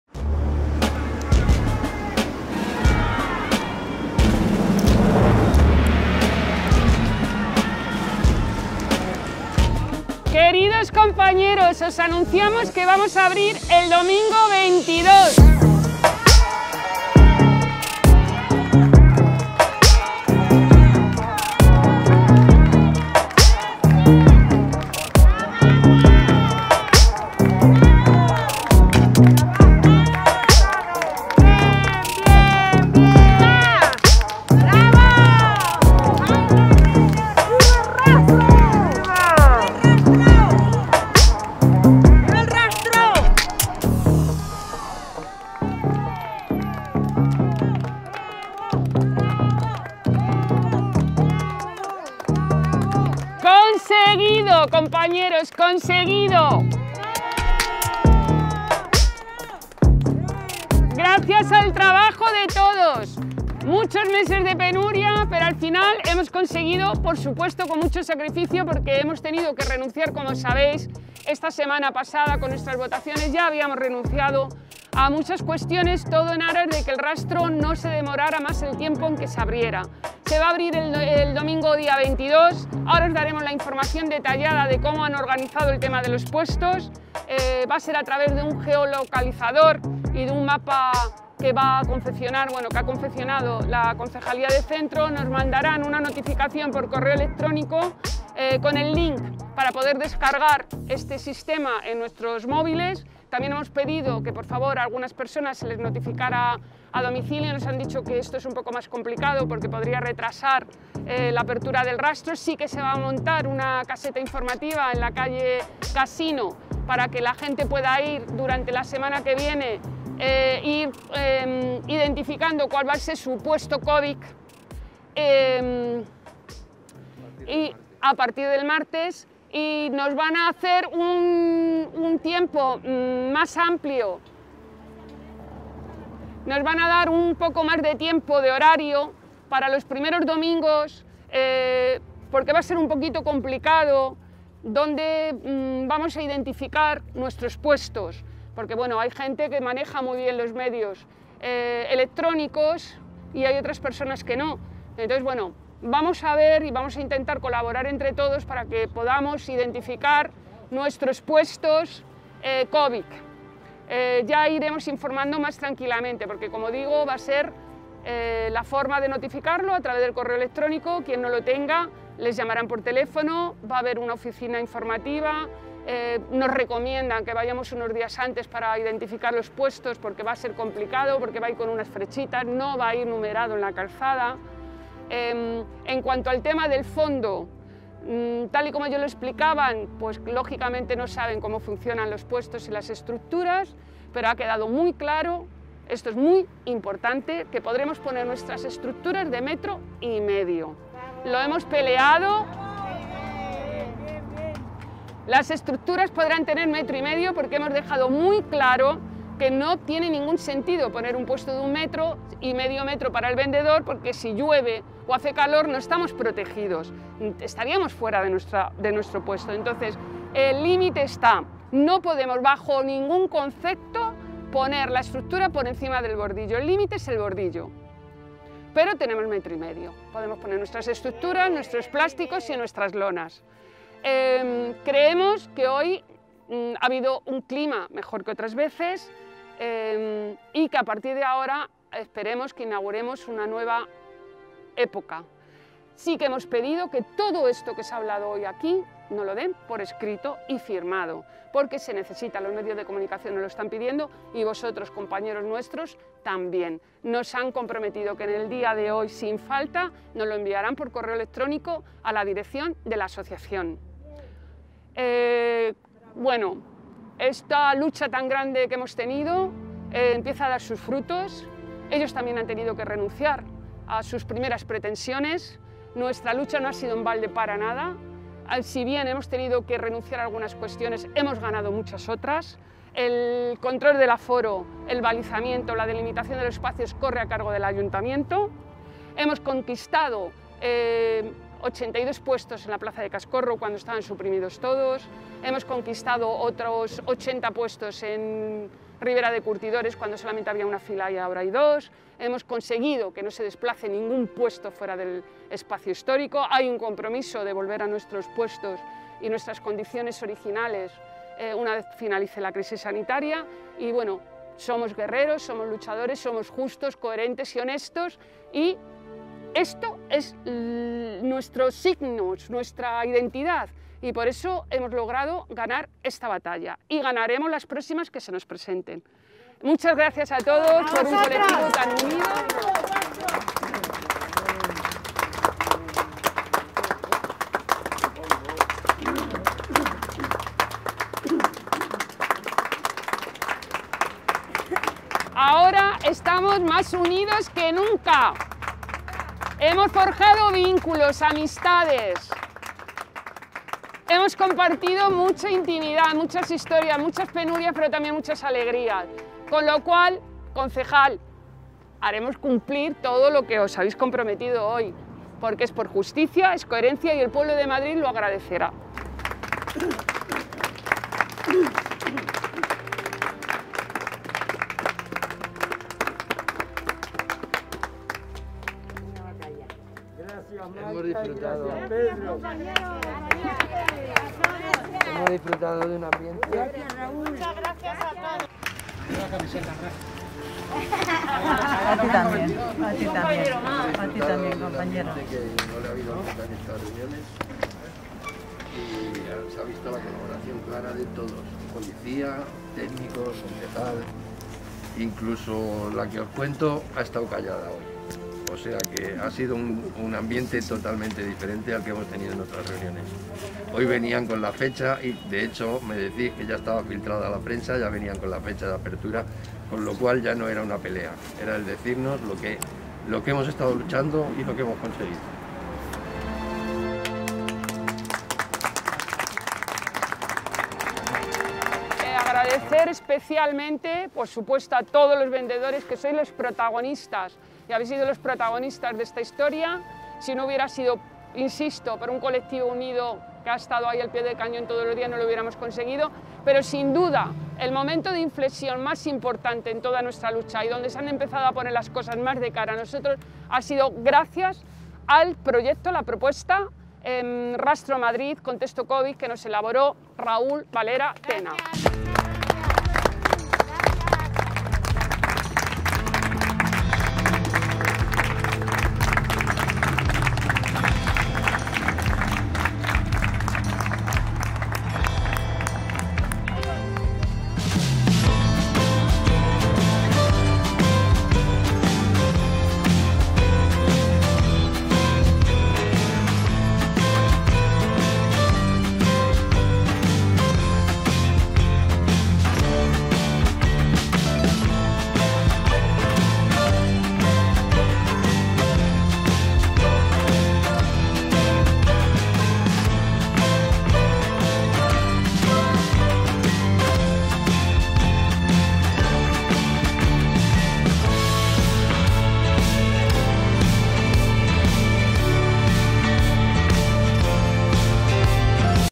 [EL RASTRO ES ASÍ] Salida del acuerdo de REAPERTURA de El Rastro para el 22 de Noviembre de 2020
Tras 8 meses de lucha por fin los vendedores ambulantes de El Rastro de Madrid llegan a un acuerdo de reapertura del famoso mercado tras haber sido cerrado debido a la pandemia del Coronavirus del 2020.

Tu imagen es importante.
Tu marca la mejor herramienta.